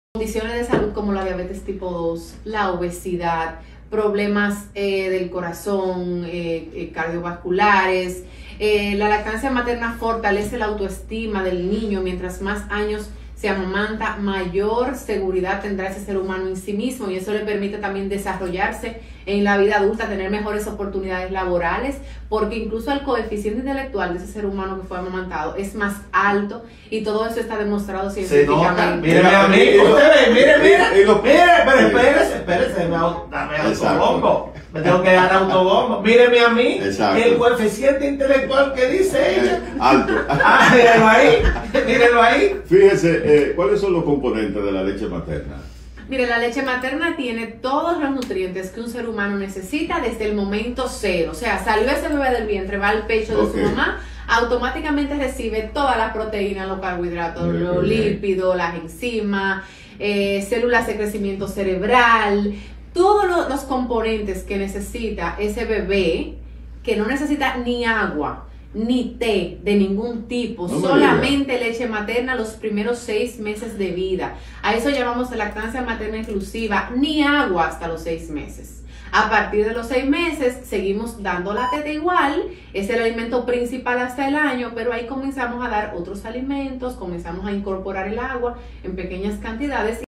Conversando con @prolactarrd sobre la leche materna
Estamos en vivo @traenvivo conversando con @prolactarrd sobre la leche materna 🤱